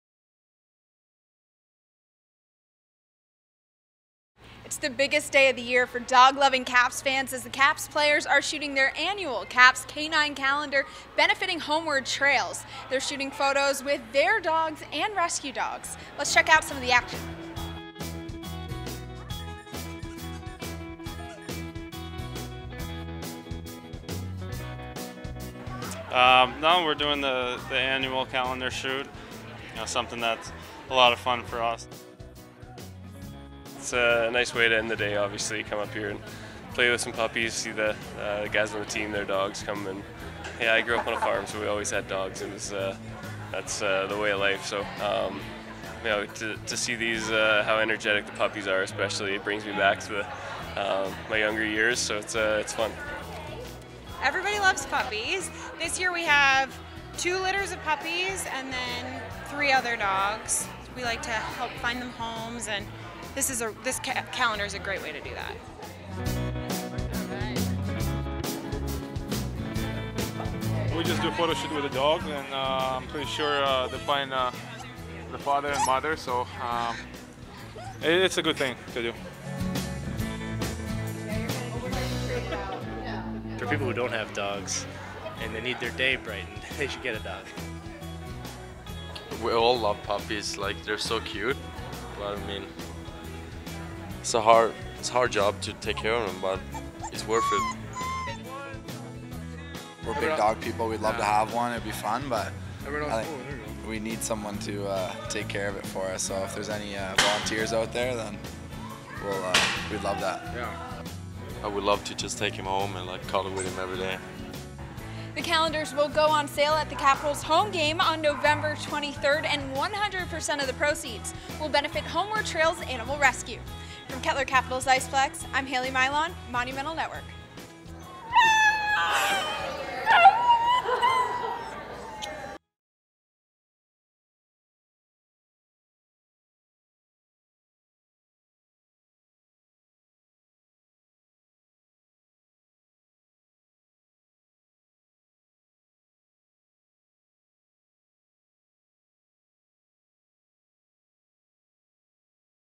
2016 Caps Canine Calendar Photo Shoot
The Washington Capitals take pictures with some of their dogs and some from Homeward Trails Animal Rescue for the 2016 Canine Calendar.